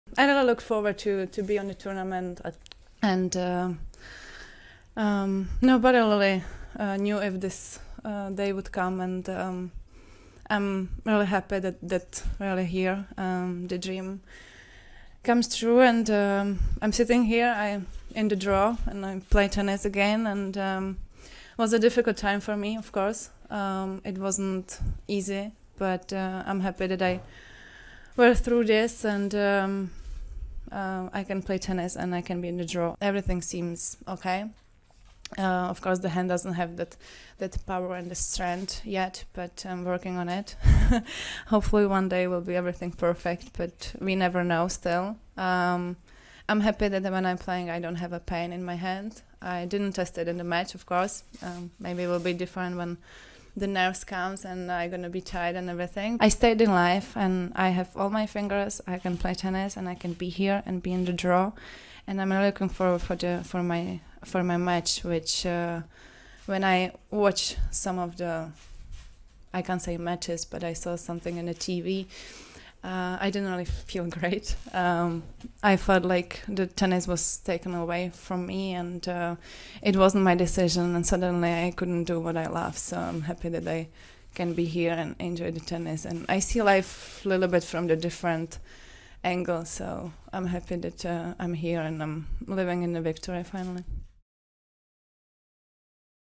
French Open return a 'dream come true' for Petra Kvitova
Petra Kvitova says it is a 'dream come true' to be making her return to tennis at the French Open after being stabbed during a burglary at her home in December. The two-time Wimbledon champion suffered serious hand injuries in the attack and has not played competitively since the incident